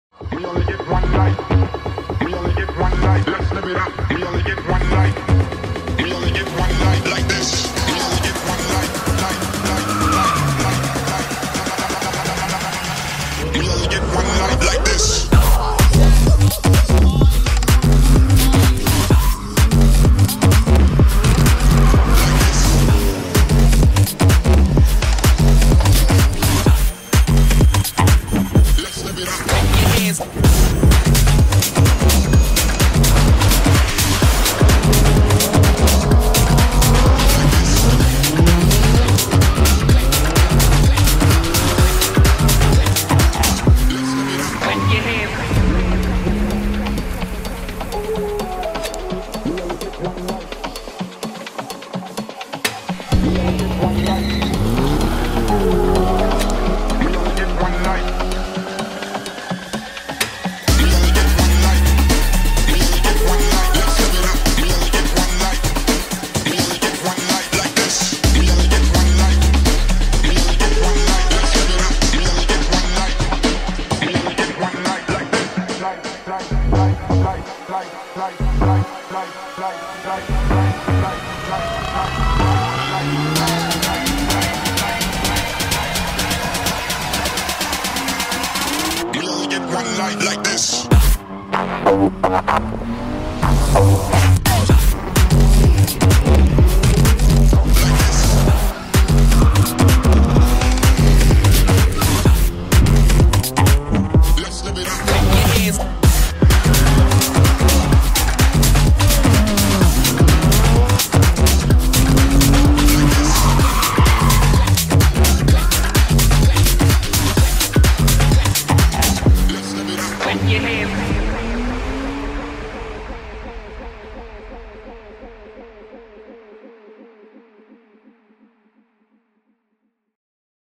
Das Auto Show Highlights 2021
The German car show comes to Santa Pod for Audi, BMW, Mercedes, VW, Porsche and more. With German only RWYB plus, Show & Shine, and more.